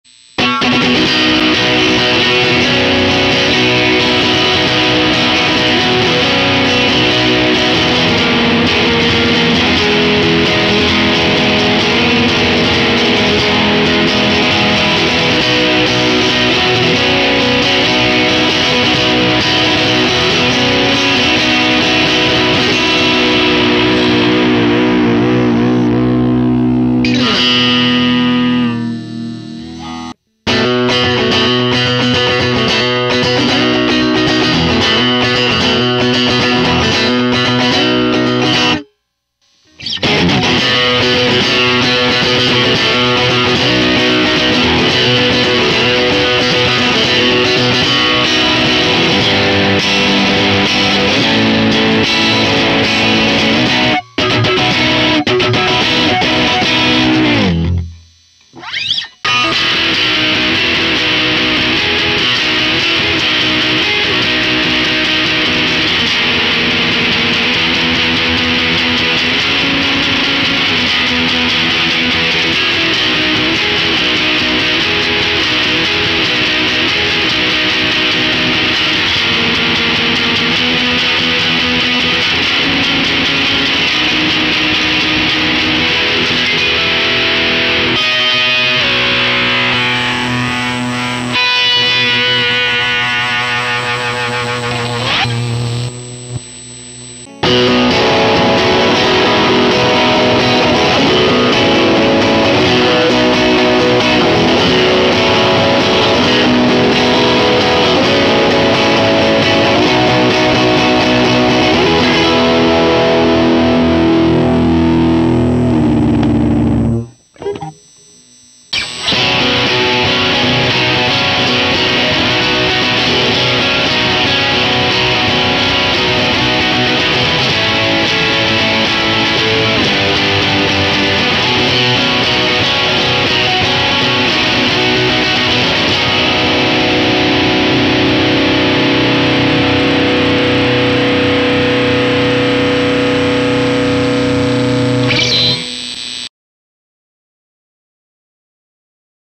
🥊Straight Punch!! Boss DS-1 (no talking)
Is there a guitarist who has never used this? Boss DS-1(1978) has became a classic!
Really nice tone for the even low gain😇.

00:00 intro
00:02 Straight Punch /Tone 50% /Level 80% /Dist 50%
00:32 Road Runner /OFF
00:40 Road Runner /Tone 50% /Level 80% /Dist 20%
01:00 Buzz Fuzz /Tone 100% /Level 80% /Dist 100%
01:38 Classic Tube /Tone 25% /Level 80% /Dist 50%
01:59 Distortion /Tone 50% /Level 60% /Dist 100%